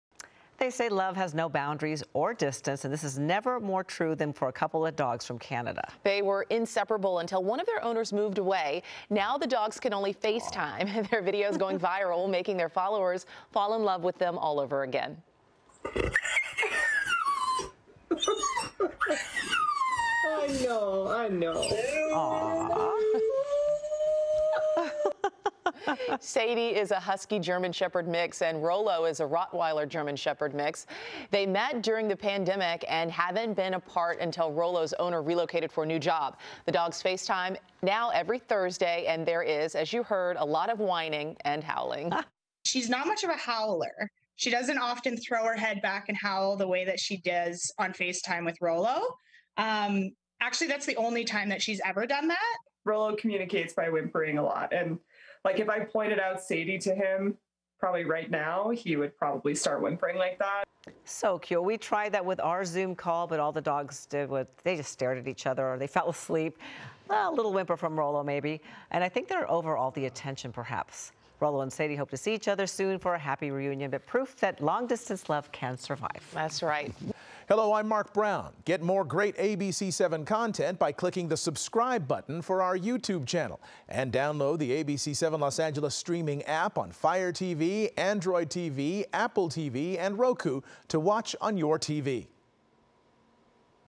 Doggie best friends stay in touch via FaceTime
Two canine best friends who now live miles apart keep in touch by FaceTime every week, howling and whimpering every time they recognize each other on the screen.